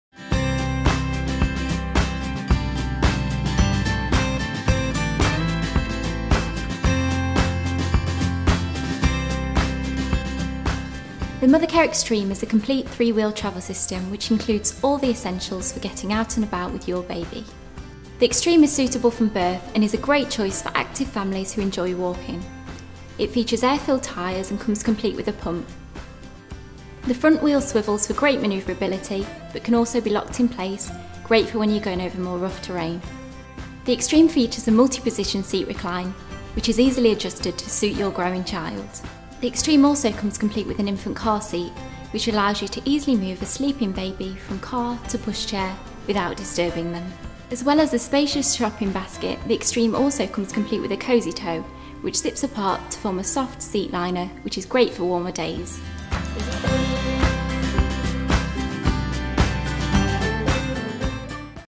Mothercare Xtreme 3 Wheel Pushchair Product Introduction Video
Mothercare Xtreme 3 Wheel Pushchair introduction video with presenter by Basement Photographic.